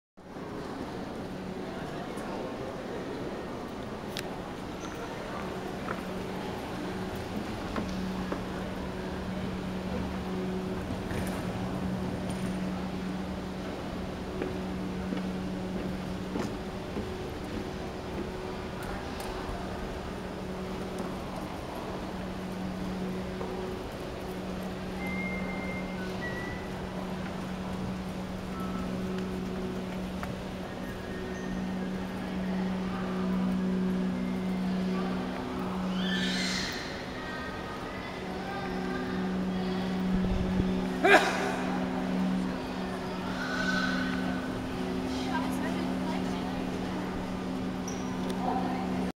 成田空港第２ターミナル内のシャトル 乗車口付近の様子
2010年10月9日